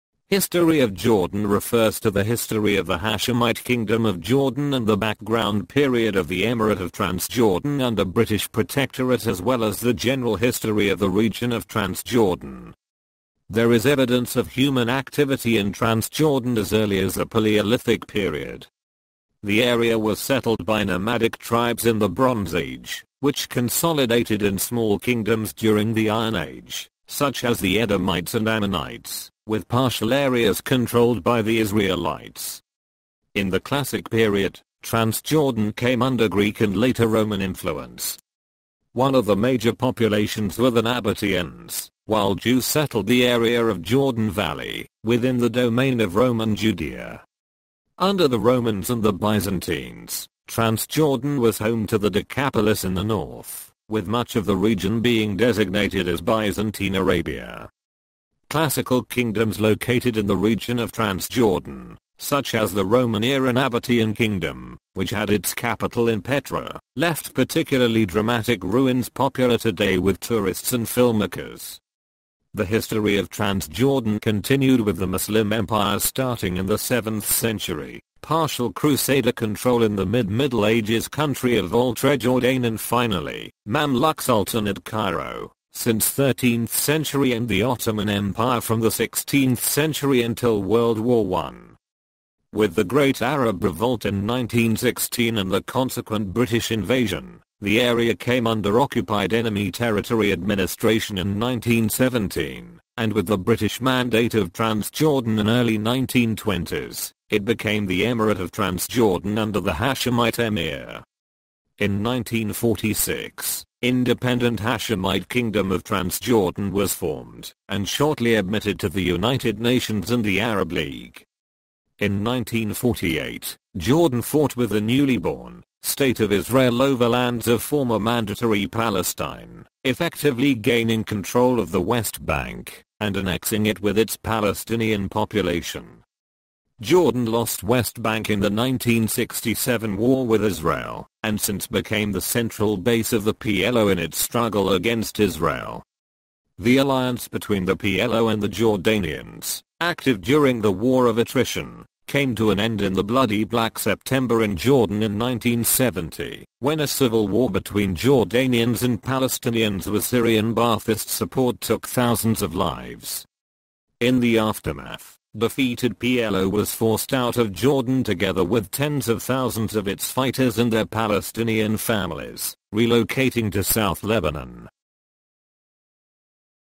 History of Jordan
History of Jordan refers to the history of the Hashemite Kingdom of Jordan and the background period of the Emirate of Transjordan under British protectorate as well as the general history of the region of Transjordan.

There is evidence of human activity in Transjordan as early as the Paleolithic period. The area was settled by nomadic tribes in the Bronze Age, which consolidated in small kingdoms during the Iron Age - such as the Edomites and Ammonites, with partial areas controlled by the Israelites. In the classic period, Transjordan came under Greek and later Roman influence. One of the major populations were the Nabateans, while Jews settled the area of Jordan Valley, within the domain of Roman Judea. Under the Romans and the Byzantines, Transjordan was home to the Decapolis in the North, with much of the region being designated as Byzantine Arabia. Classical kingdoms located in the region of Transjordan, such as the Roman-era Nabatean kingdom, which had its capital in Petra, left particularly dramatic ruins popular today with tourists and filmmakers. The history of Transjordan continued with the Muslim empires starting in the 7th century, partial crusader control in the mid-Middle Ages (country of Oultrejordain) and finally, Mamluk Sultanate (Cairo) since 13th century and the Ottoman Empire from the 16th century until World War I.